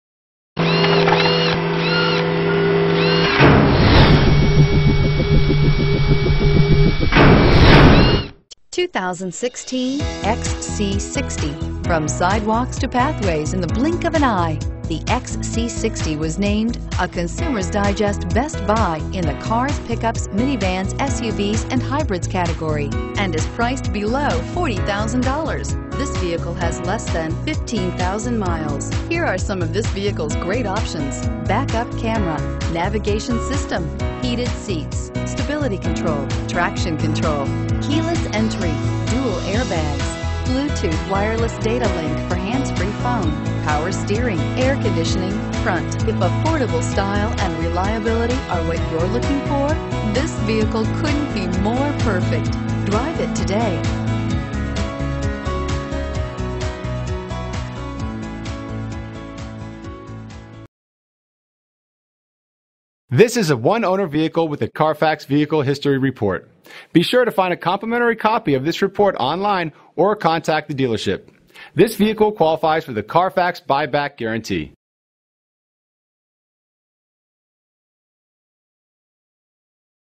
2016 Volvo XC60 used, Mazda 3, Prius, Sienna, Grand Cheoroke, Impreza, Burlingame, Bay Area, Penins
2016 Volvo XC60 T5 Drive-E Premier

Putnam Automotive
3 California Drive

Looking for the right suv? Today could be your lucky day. With 13222 miles, this MAGIC BLUE METALLIC, 2016 Volvo XC60 equipped with Automatic transmission could be yours. Request more information and set up a test drive right away.